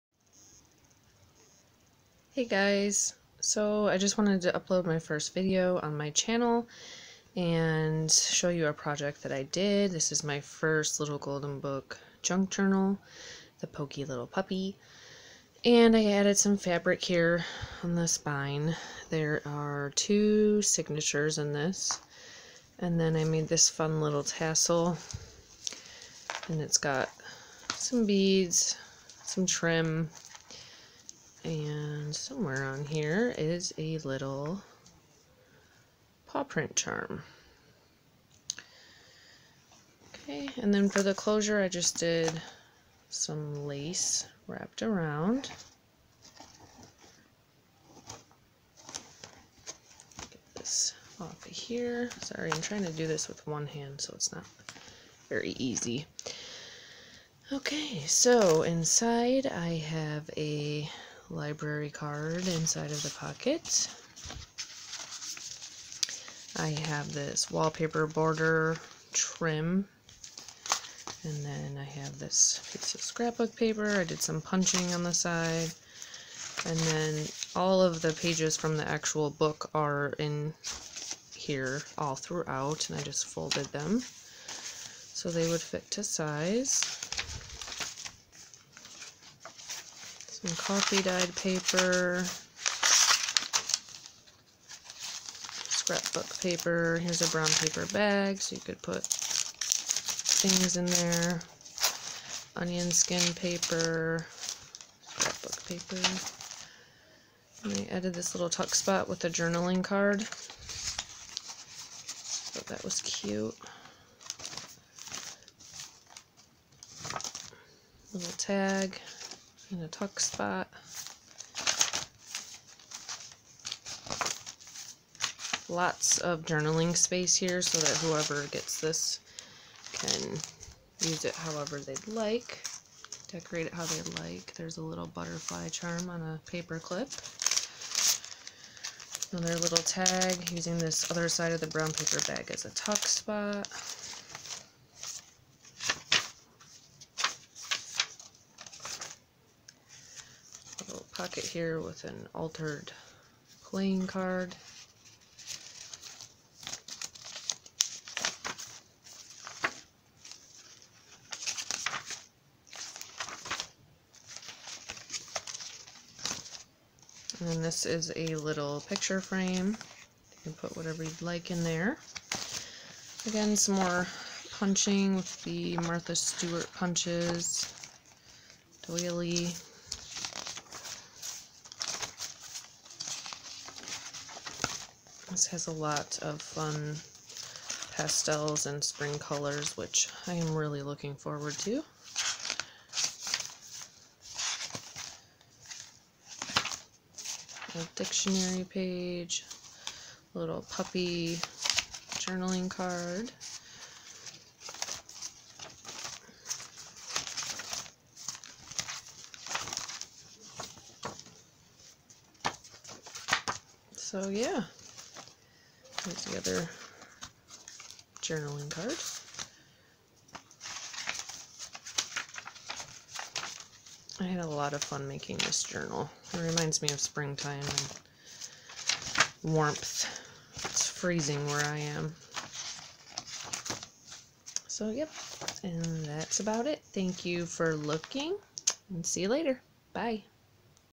Junk Journal Flip Through Little Golden Book The Poky Little Puppy
Please enjoy this flip through of my first Little Golden Book Junk Journal - The Poky Little Puppy.

It has 2 signatures, and a lot of open journaling space for the recipient to use to write in or decorate and embellish however they would like.

It has a nice Spring pastel color scheme as well.

🔔🔔Link to purchase on Etsy